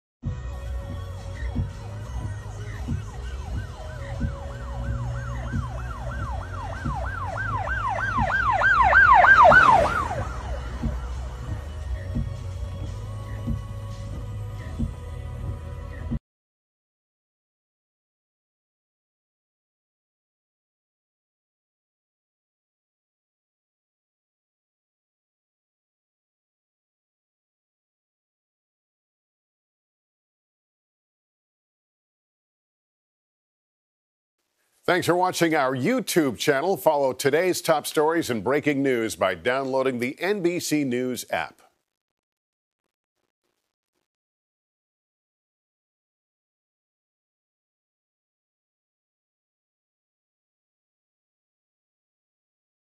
Video Shows Burning Power Line, Falling Traffic Light After Ian
Emergency services in parts of Florida struck by storm Ian issued the same warning early Thursday: Stay off the roads.